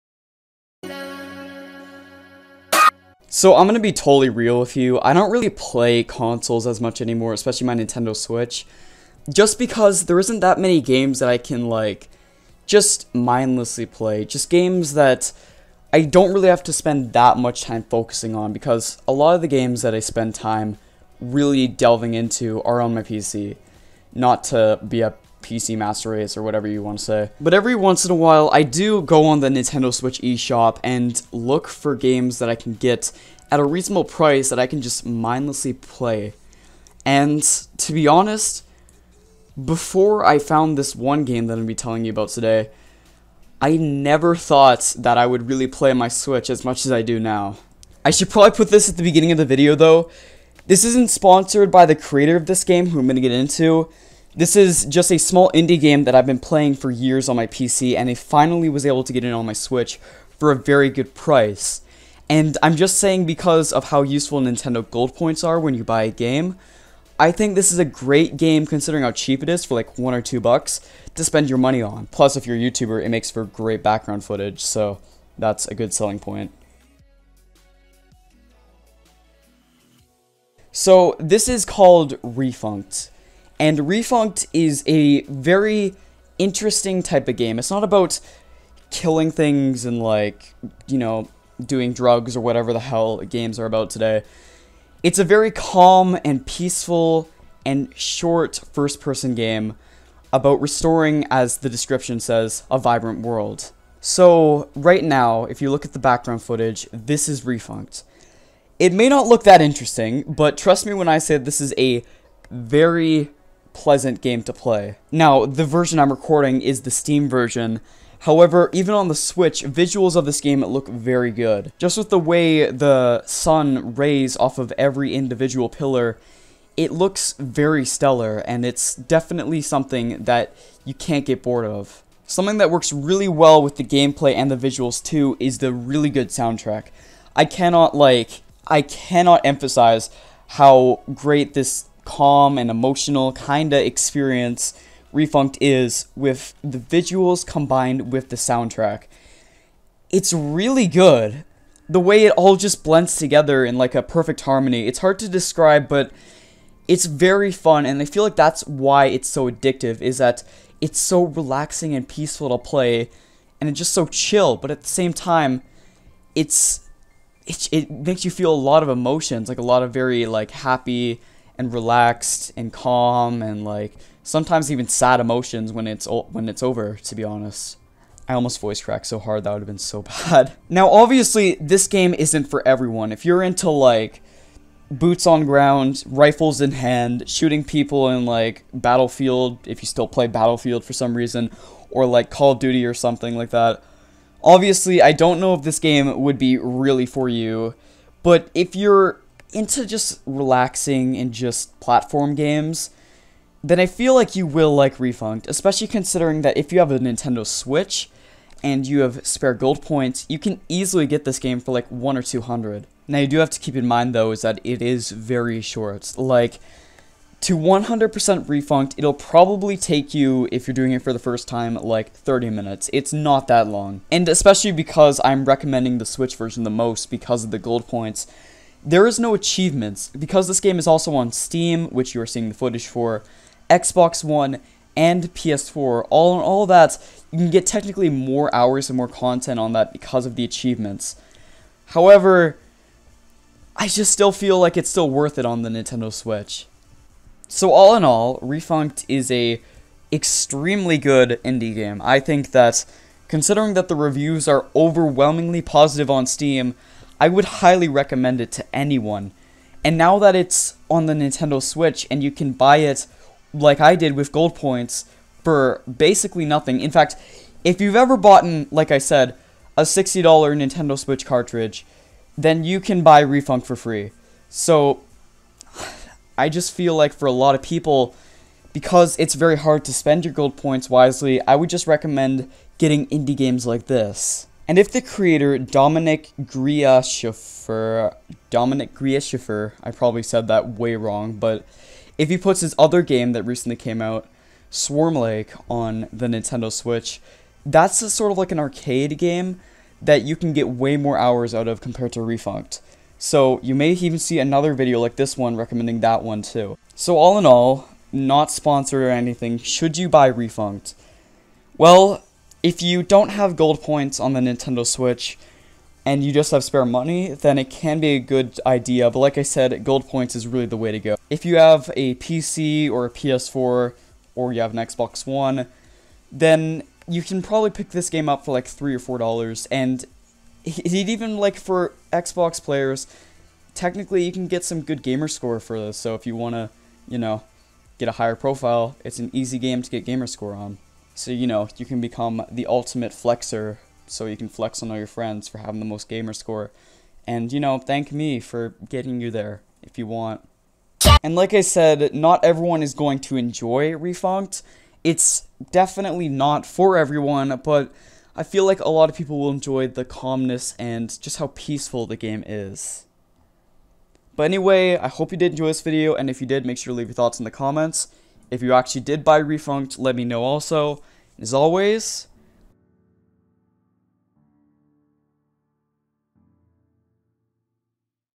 The Very Cheap Nintendo Switch Game You SHOULD Play! (PS4, Xbox One, Steam)
There's been this game I've been adoring for years, and it's now finally on the Switch.. and it's really cheap. I'll give you a rundown on what this game is about.